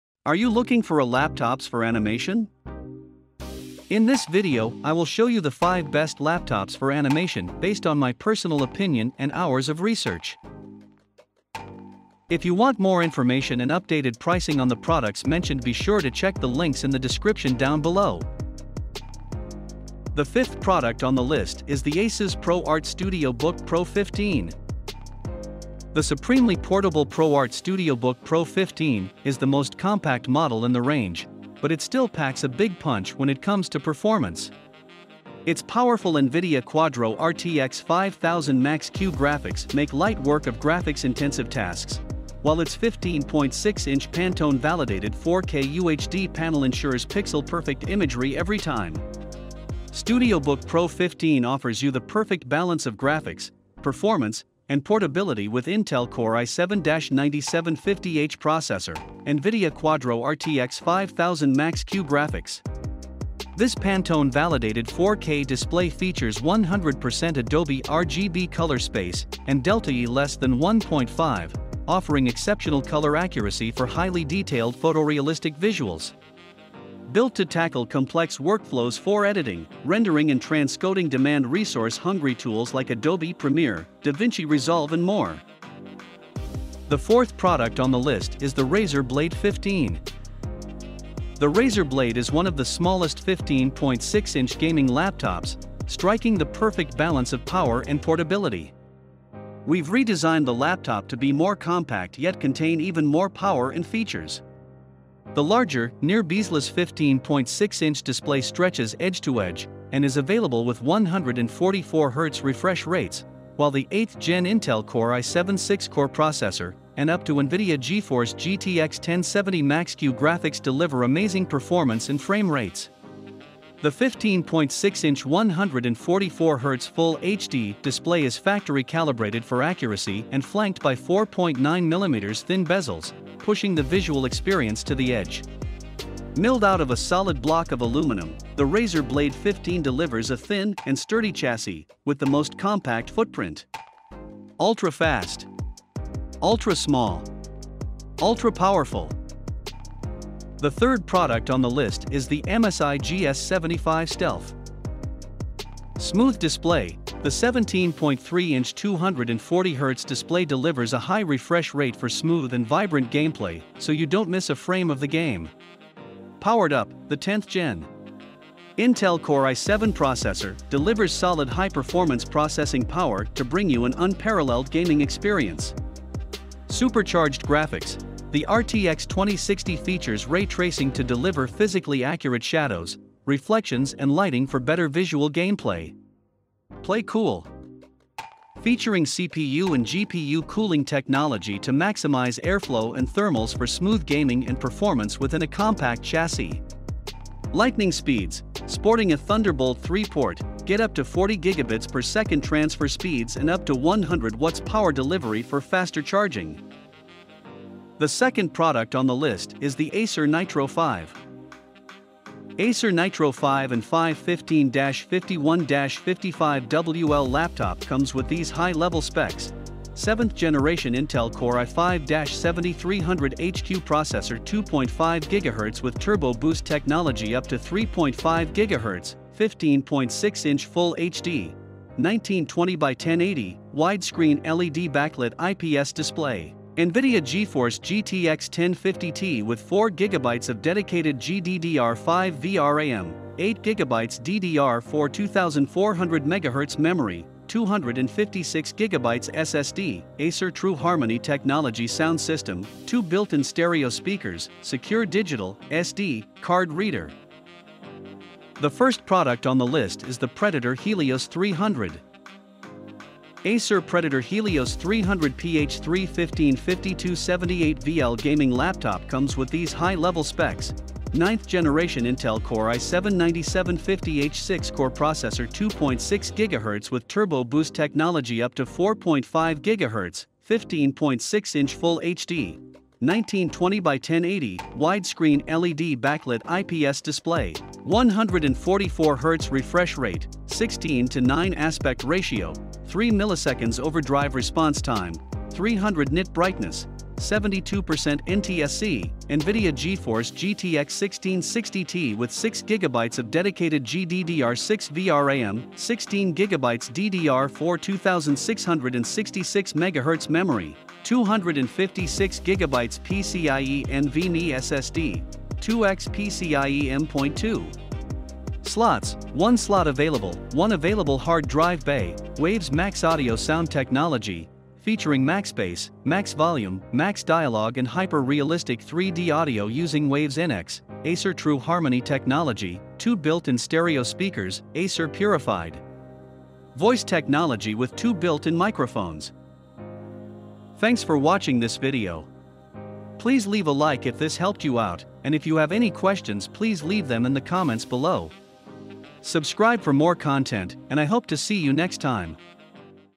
TOP 5: Best Laptops For Animation | Laptops For Animators
5. Asus pro art studio book pro 15 (Amazon)
4. razer blade 15 (Amazon)
3.  MSI gs75 stealth (Amazon)
2. Acer nitro 5 (Amazon)
1. predator helios 300 (Amazon)

Are you looking for the Best Laptops for Animation? In this video I will show you the best laptops for animation.